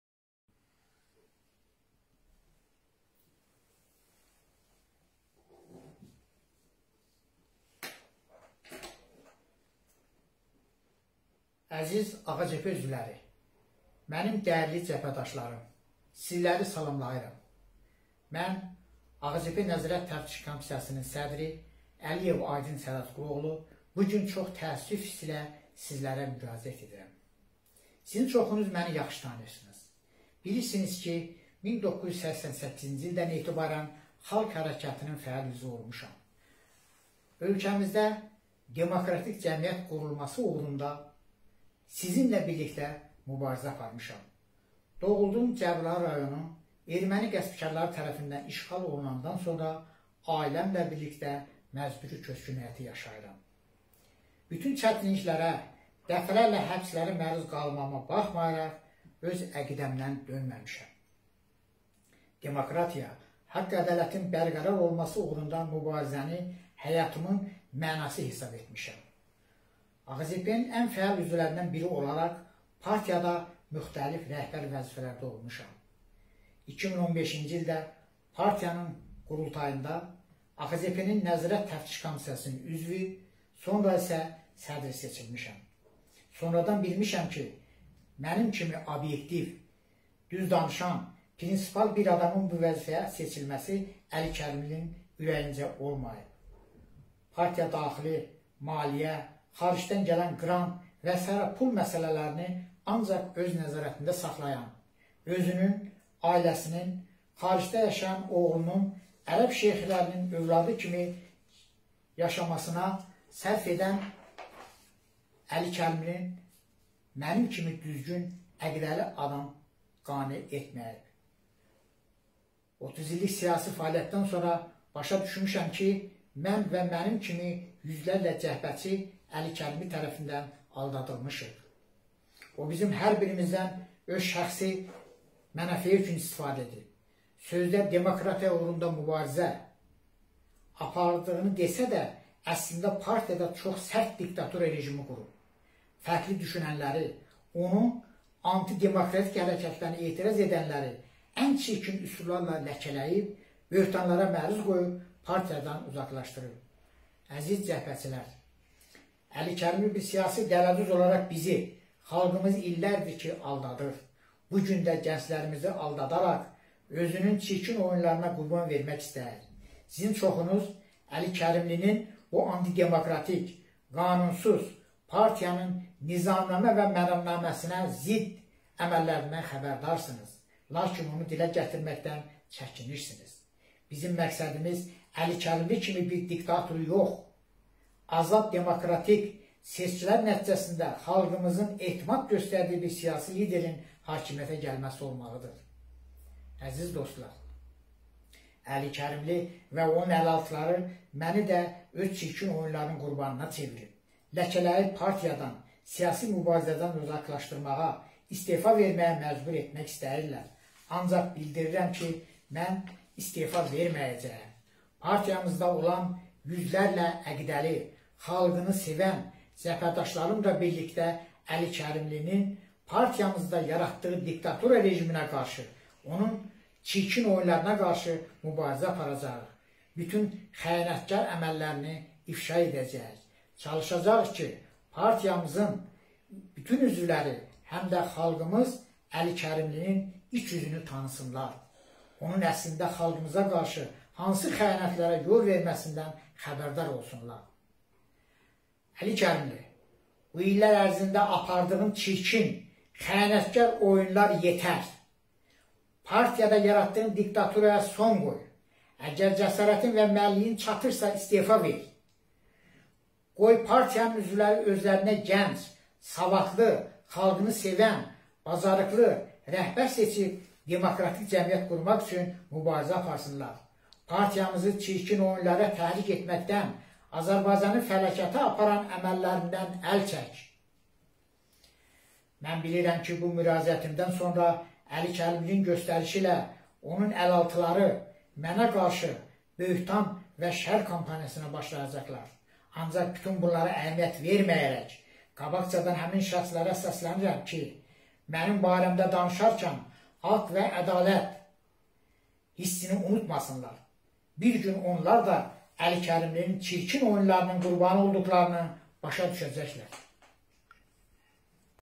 AXCP də qiyam: NTK sədri Əli Kərimlini istefaya çağırdı
Azərbaycan Xalq Cəbhəsi Partiyasının (AXCP) Nəzarət-Təftiş Komissiyasının (NTK) sədri Aydın Əliyev partiya üzvlərinə müraciət edib.
O, müraciətində bəyan edib ki, yüzlərlə cəbhəçi Əli Kərimli tərəfindən aldadılıb.
A.Əliyev, həmçinin müraciətində Əli Kərimlini istefaya çağırıb: “2015-ci ilə AXCP-nin qurultayında partiyanın Nəzarət Təftiş Komitəsinin üzvü, daha sonra sədri seçilmişəm. Sonradan bilmişəm ki, mənim kimi obyektiv, düz danışan, prinsipial adamın bu vəzifəyə seçilməsi Əli Kərimlinin ürəyincə olmayıb. Partiyadaxili maliyyə, xaricdən gələn qrant və.s pul məsələlərini ancaq öz nəzarətində saxlayan, özünün, ailəsinin, xaricdə yaşayan oğlunun ərəb şeyxlərinin övladı kimi yaşamasına sərf edən Əli Kərimlini mənim kimi əqidəli, düzgün adam qaneə etməyib”.
AXCP üzvü qeyd edib ki, Əli Kərimli partiyada çox sərt diktatura rejimi qurub: “30 illik siyasi fəaliyyətdən sonra başa düşmüşəm ki, mən və mənim kimi yüzlərlə cəbhəçi Əli Kərimli tərəfindən aldadılıb. O bizim hər birimizdən öz şəxsi mənafeyi üçün istifadə edib. Sözdə demokratiya uğrunda mübarizə aparıldığını desə də, əslində partiyada çox sərt diktatura rejimi qurub. Fərqli düşünənləri, onun antidemokratik hərəkətlərinə etiraz edənləri ən çirkin üsullarla ləkləyib, böhtanlara məruz qoyub, partiyadan uzaqlaşdırıb.

Əli Kərimli siyasi dələduz olaraq bizi və xalqımızı illərdir aldadır. Bu gün də gənclərimizi aldadaraq özünün çirkin əməllərinə qurban vermək istəyir. Cəbhəçilərin çoxu Əli Kərimlinin antidemokratik, qanunsuz, partiyanın nizamnamə və məramnaməsinə zidd əməllərindən xəbərdardır. Lakin bunu dilə gətirməkdən çəkinirlər”.

A.Əliyev əlavə edib ki, Əli Kərimli və onun əlaltıları partiyada hamını öz çirkin oyunlarının qurbanına çevirib: “Məni ləkələyib, siyasi mübarizədən uzaqlaşdırmağa, istefa verməyə məcbur etmək istəyirlər. Ancaq mən istefa verməyəcəm. Partiyamızda olan yüzlərlə əqidəli, xalqını sevən cəbhədaşlarımla birlikdə Əli Kərimlinin partiyamızda yaratdığı diktatura rejiminə, çirkin oyunlarına qarşı mübarizə aparacağıq. Bütün xəyanətkar əməllərini ifşa edəcəyik. Çalışacağıq ki, partiyamızın bütün üzvləri, xalqımız Əli Kərimlinin iç üzünü tanısınlar. Onun əslində xalqımıza qarşı hansı xəyanətlərə yol verməsindən xəbərdar olsunlar”.

Partiya üzvü Əli Kərimlidən illər ərzində apardığı çirkin və xəyanətkar oyunlara son qoymağa çağırıb: “Yetər. Partiyada yaratdığın diktaturaya son qoy. Əgər cəsarətin və mənliyin çatırsa, istefa ver. Qoy partiyanın üzvləri özlərinə gənc, savadlı, xalqını sevən, bacarıqlı rəhbər seçsinlər. Partiyamızı çirkin oyunlara təhrik etməkdən, Azərbaycanı fəlakətə aparan əməllərindən əl çək”.

Bundan əlavə, A.Əliyev ona qarşı böhtan kampaniyasına başlanılacağının anonsunu verib: “Bilirəm ki, bu müraciətimdən sonra Əli Kərimlinin göstərişi ilə onun əlaltıları mənə qarşı böhtan, şər kampaniyasına başlayacaqlar. Ancaq bütün bunlara əhəmiyyət verməyərək qabaqcadan həmin şəxslərə səslənirəm ki, mənim barəmdə danışarkən haqq və ədalət hissini unutmasınlar. Bir gün onlar da Əli Kərimlinin çirkin oyunlarının qurbanı olduqlarını başa düşəcəklər”.